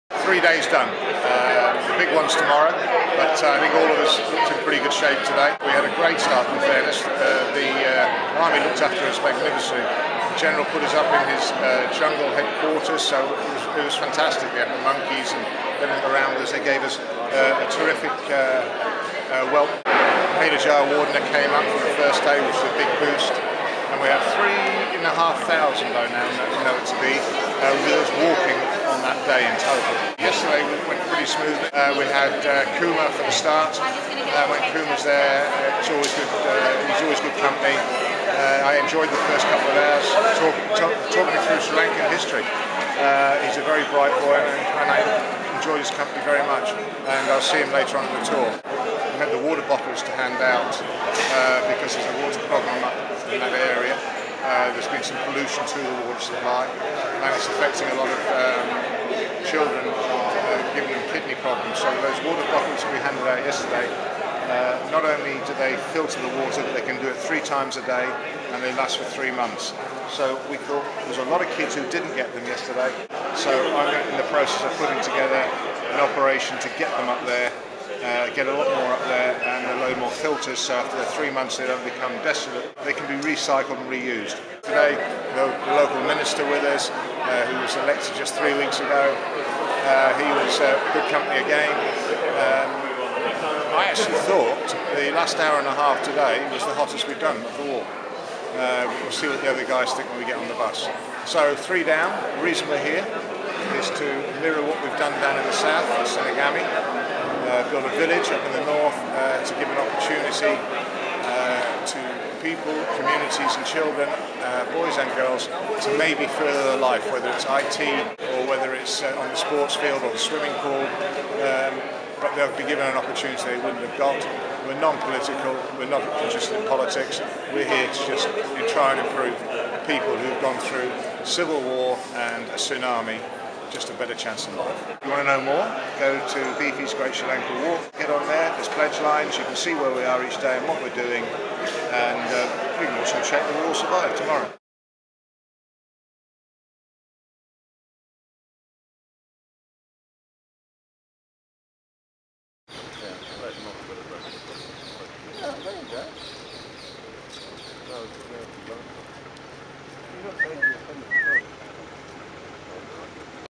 Ian Botham's Beefy Walk Video blog: Part 1
Beefy's Big Sri Lanka Walk raising funds for Laureus-supported projects completes its third day. Learn what has happened so far straight from the man himself.